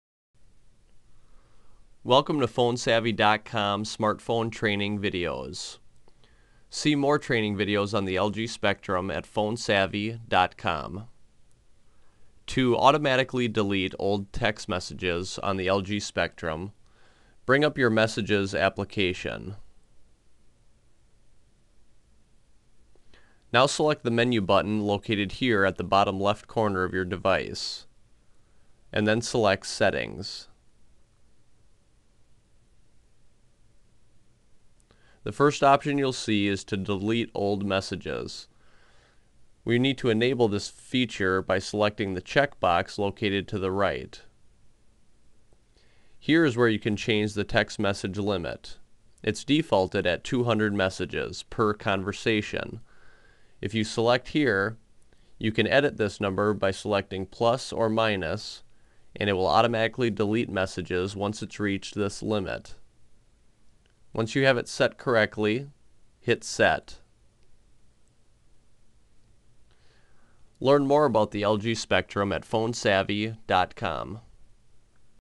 How Do I Set My LG Spectrum To Automatically Delete Old Text Messages?
This video will show how to set the LG Spectrum to delete text messages that are old to prevent needless consumption of space.